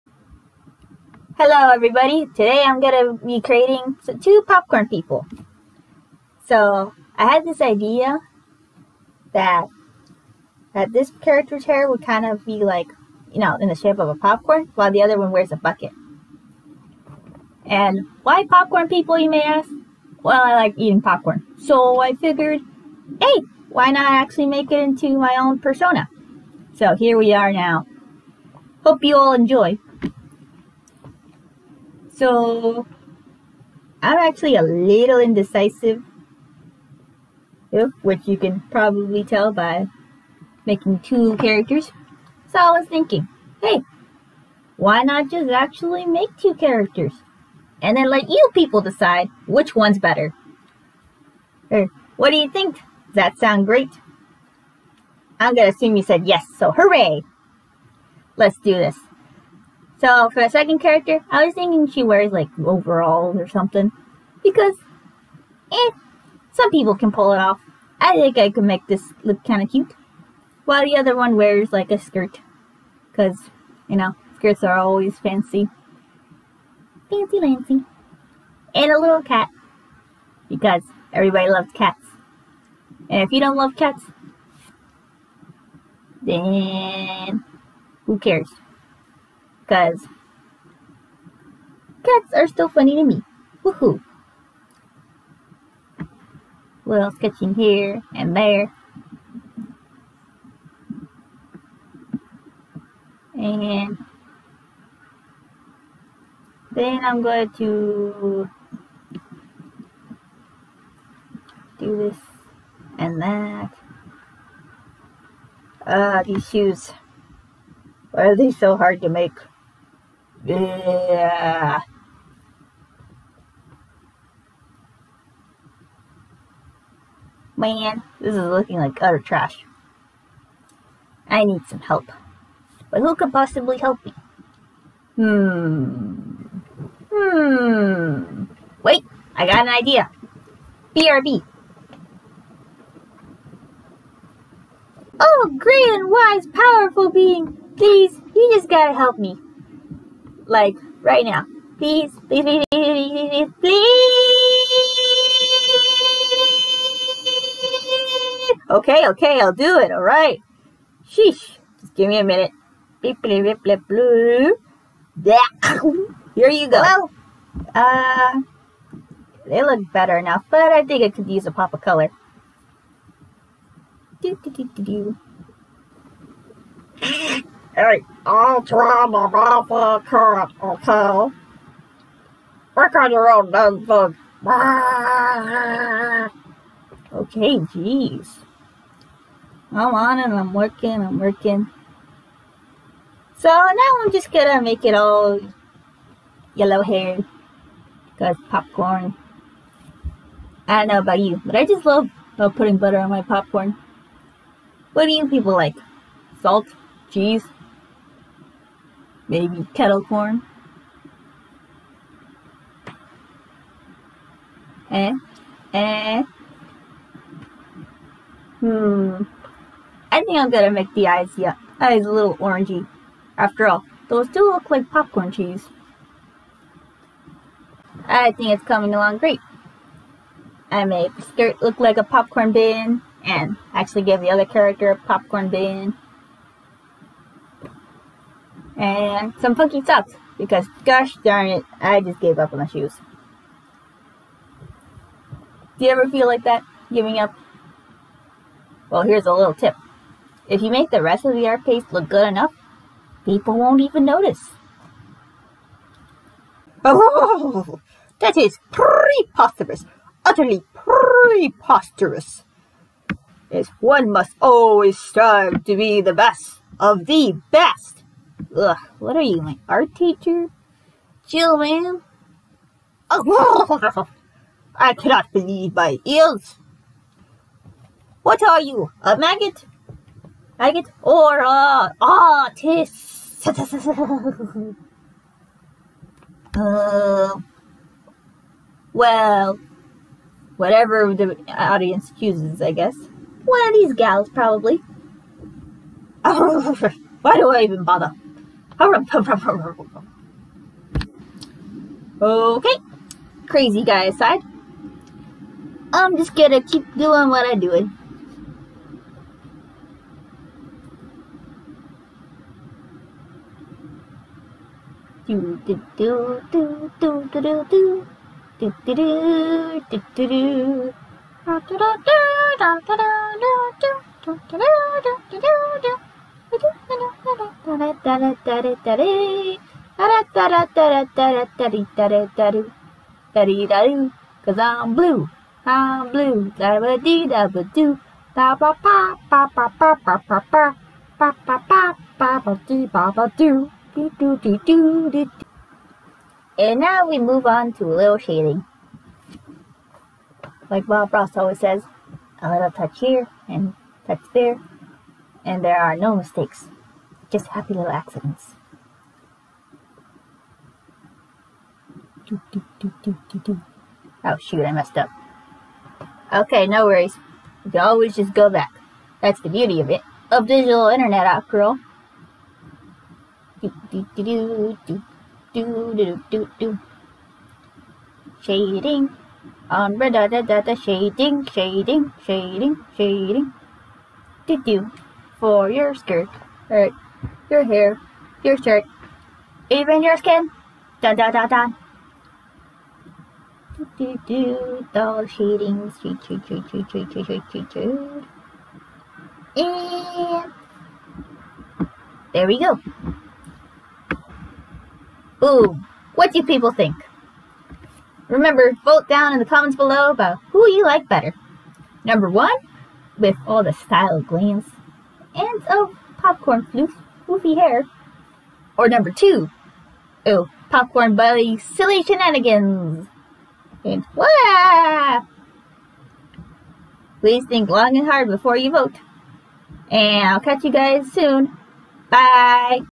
Popcorn people
I draw some creatures that live in a movie theater. They dress up base on popcorn because they think it's fashionable.
Vote for 1 or 2 in the comments for who you like better.